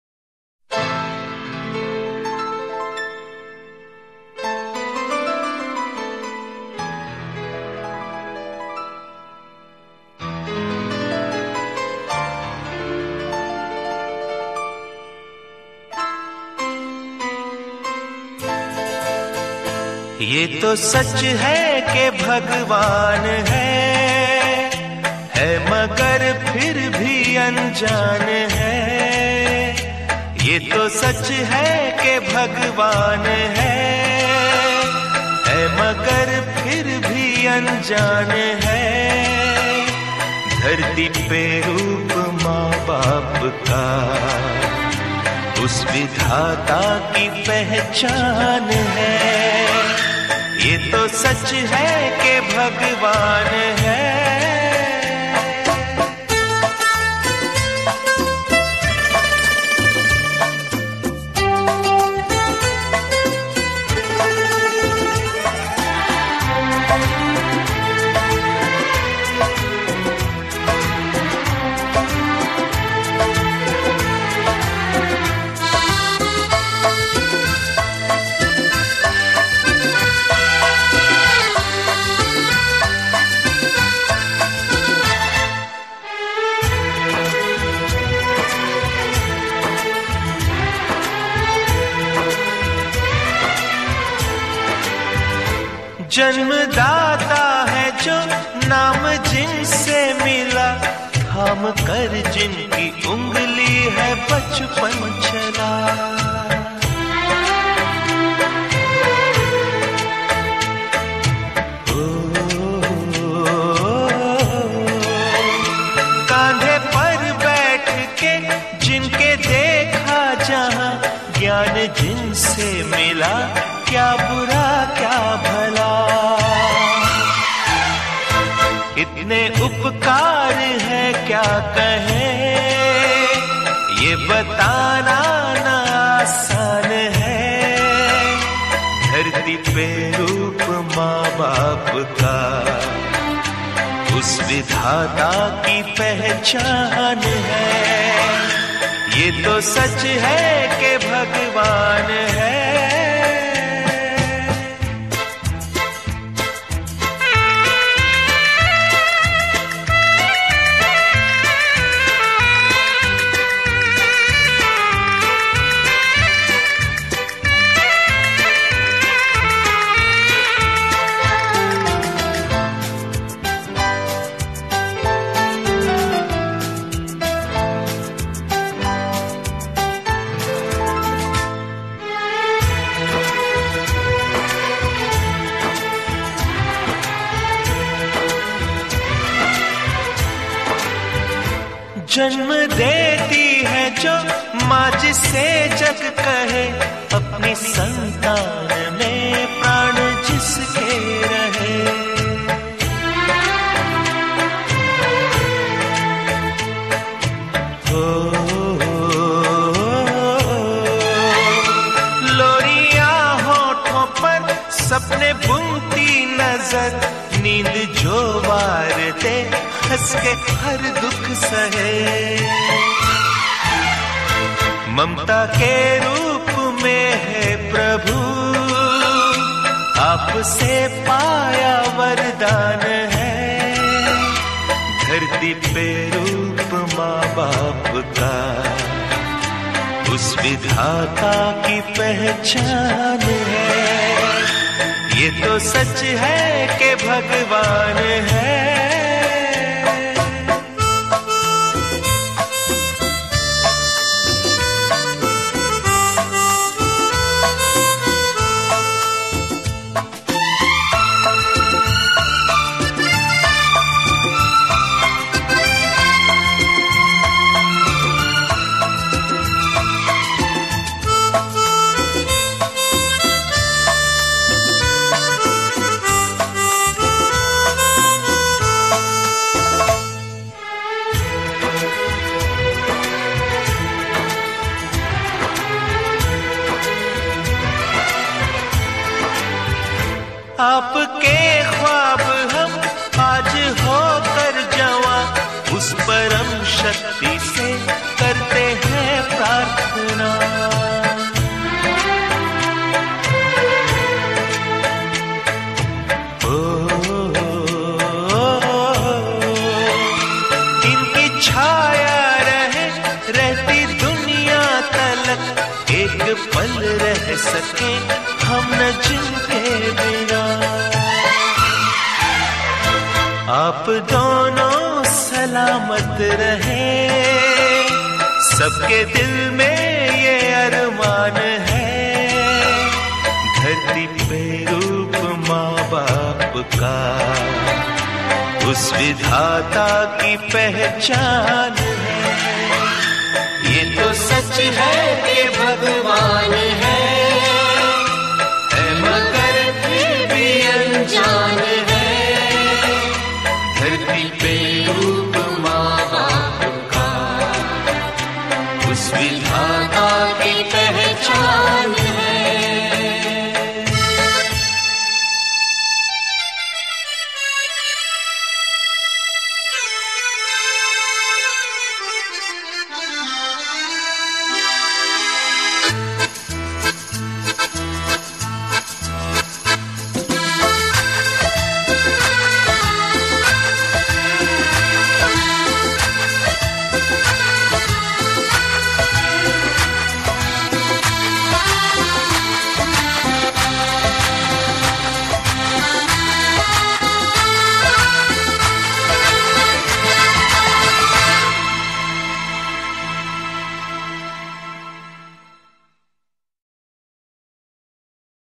HAPPY BIRTH DAY TO DEAR PAPA,,,
This video I created for my father, to congratulate him on his birthday,
I was learning video editing for the past year, this video is an example,
I have tried to do something stupendous, if you like it, please bless me.
NOTE : any video or audio from my channel is not for commercial use, but its only for fun